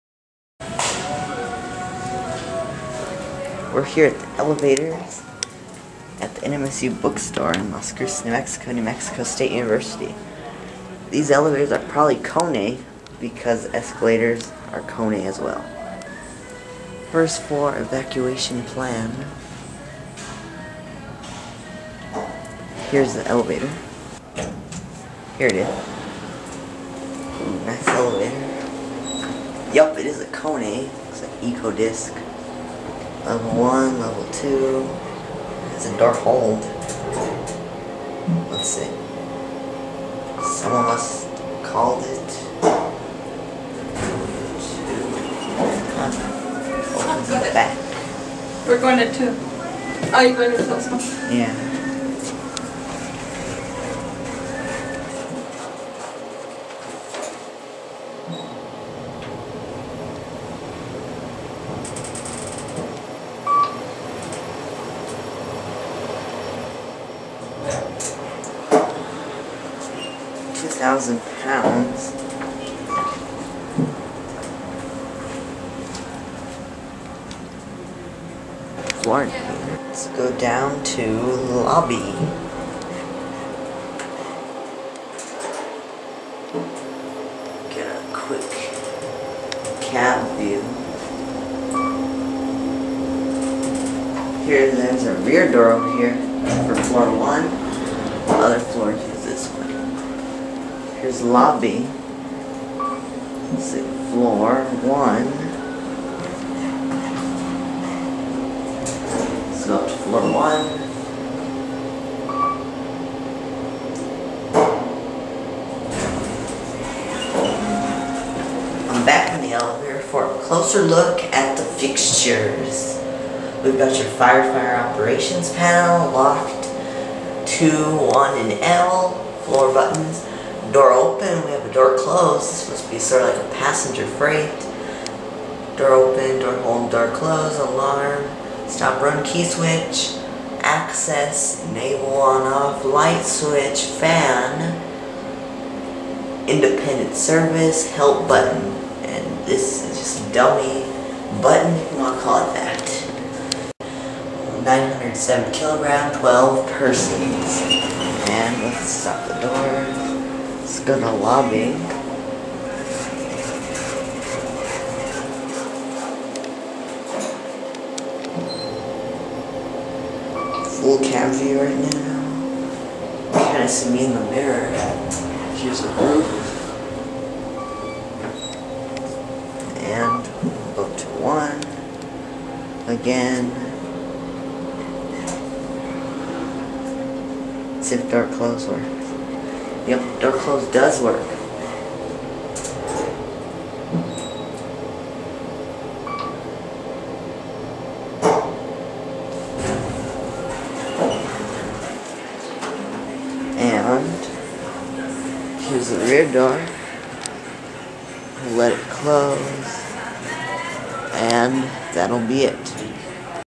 Kone EcoDisc Elevator at NMSU Bookstore
The KONE EcoSpace/Disc elevator at the new NMSU Bookstore in Las Cruces, NM.
[Filmed on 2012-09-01 with the Sony CX260V in 1080p60]
The building was built in 2011.

**TECHNICAL SPECS**
Manufacturer: Kone
Capacity: 900 kg
Landings Served: 3
Door Safety: Sensor
Type: Kone EcoDisc
Installed: 2011